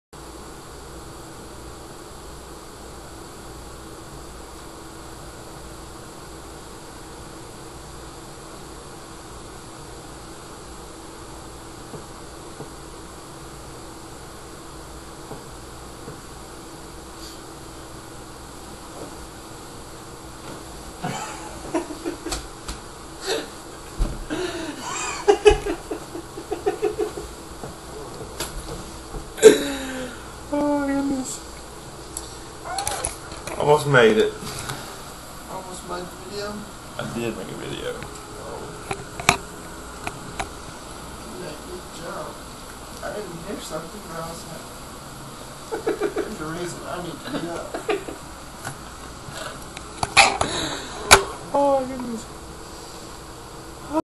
There's a reason I need to be up!?
Well I got out of class early today so I decided to come back and prank my roommate. again. However, as far as I managed to get here(sneaking into his room, turning on his desktopPC, and upload his camera), I didn't make it all the way under his bed as I had planned. But I'ts still hilarious.